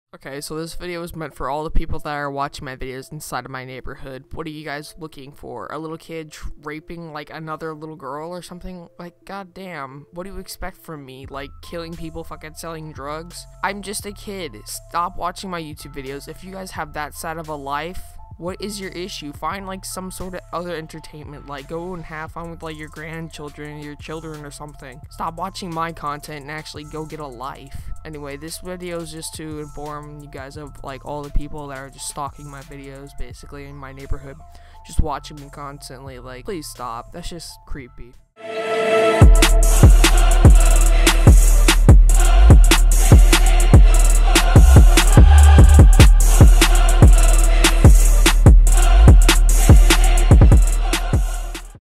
DING DONG DITCHED MY NEIGHBOR GONE WRONG
The title is click bait i know but it is just to get my stalkers/fans/viewers  in my neighborhood to watch the video so that way they know i know they are watching me.